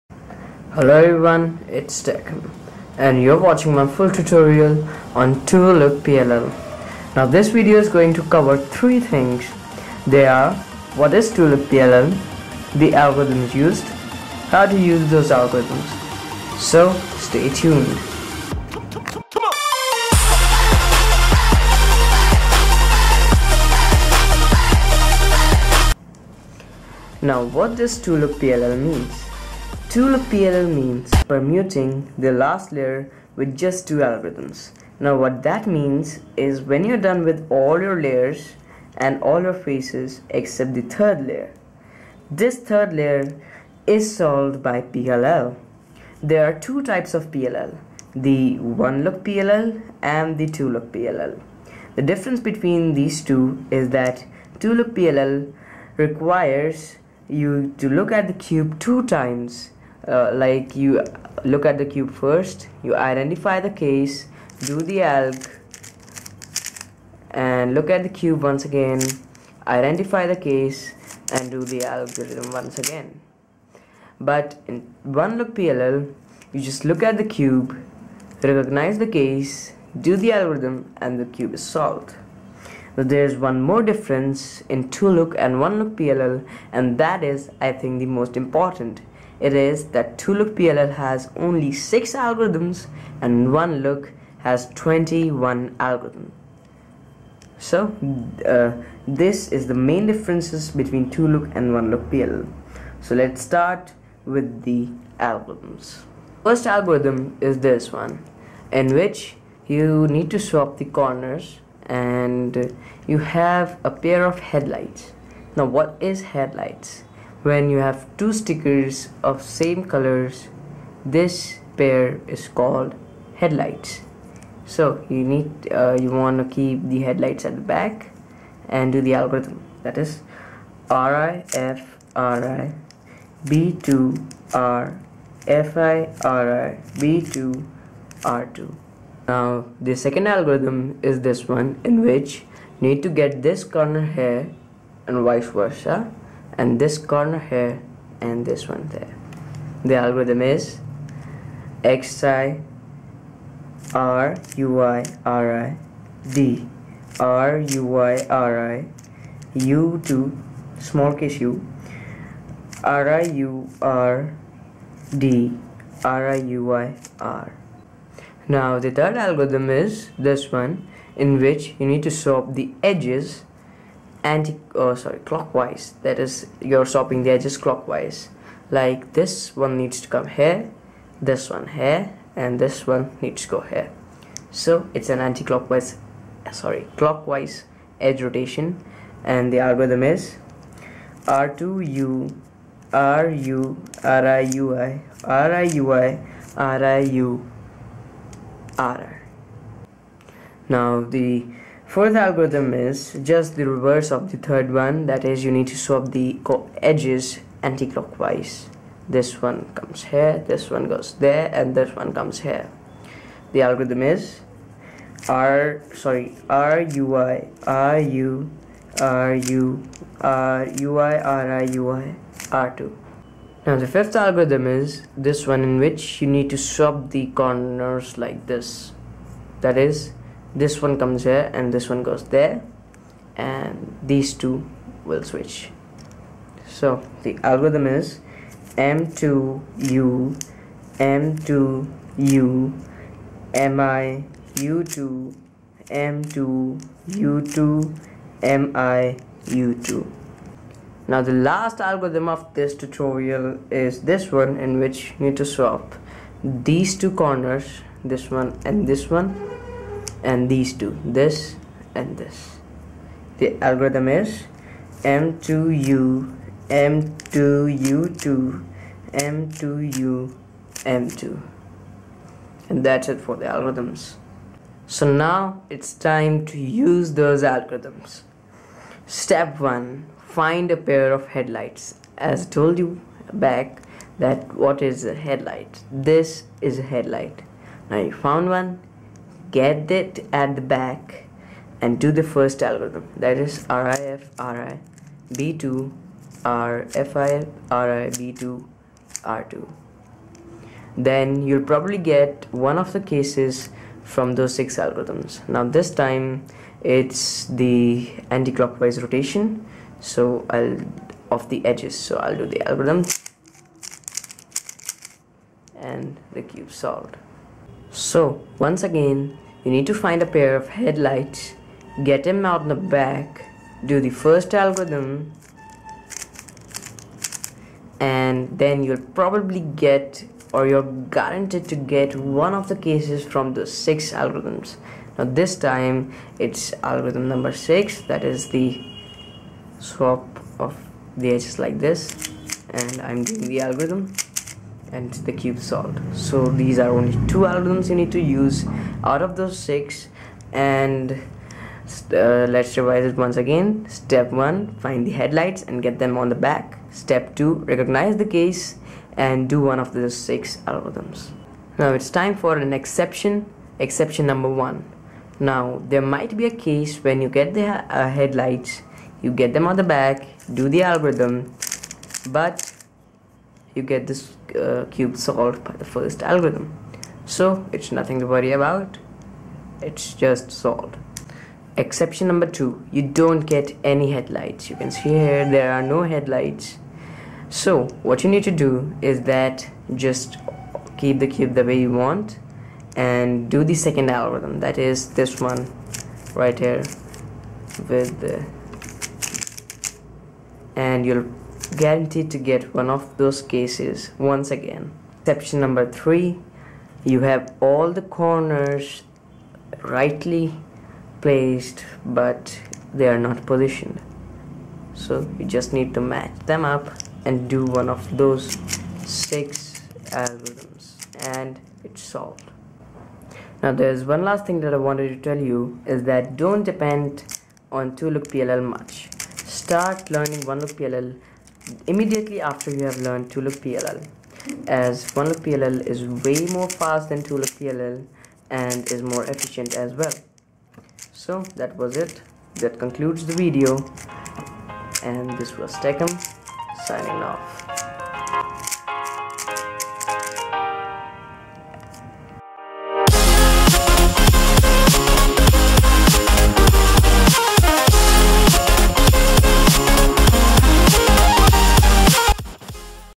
Solving The Rubik's Cube In 20 SECONDS!!!(tutorial)
Hello everyone,  this is TeCM back with another video and this time it is another tutorial but this time it's about "Permuting The Last Layer (PLL)". I hope you like it.